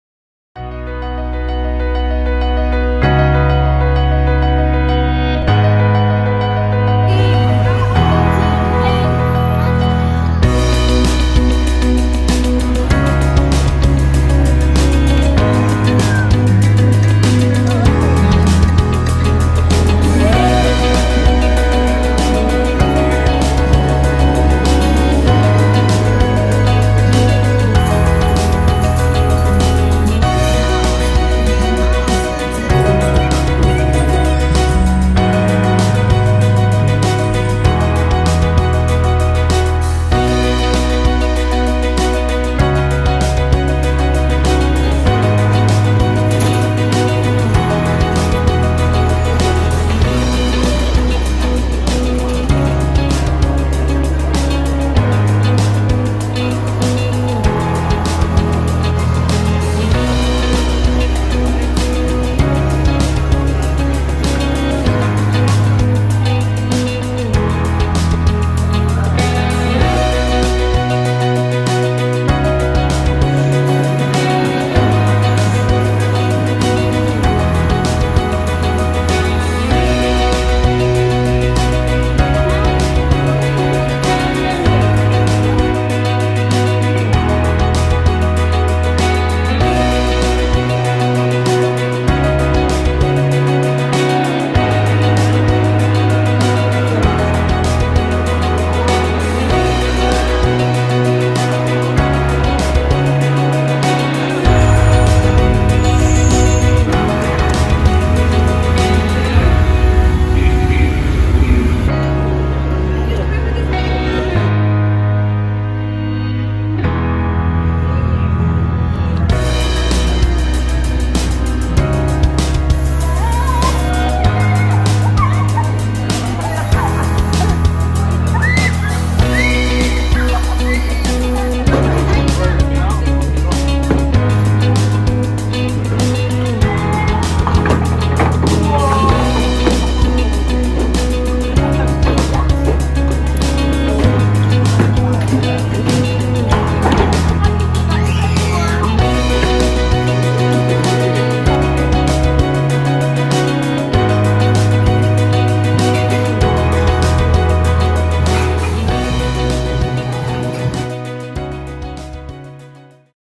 Amazing Las Vegas Family Vacation 2019
Here is a video of our last visit to Las Vegas in December 2019, we have been to Las Vegas many times but I like to think my videos are getting better :)